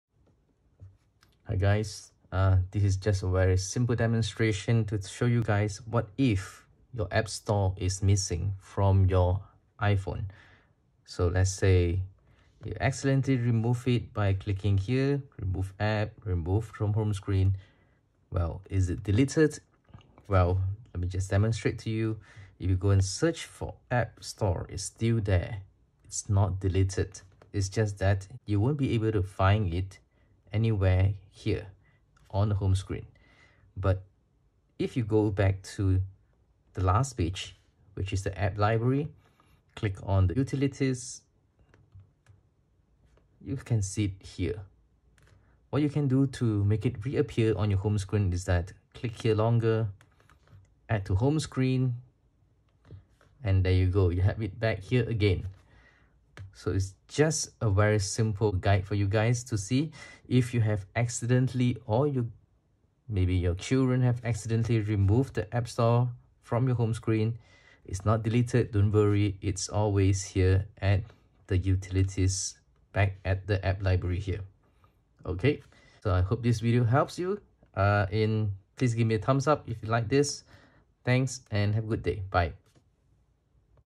How to get back App Store on my IPhone?
In this video, I will show you how to get the App Store back on your Home Screen.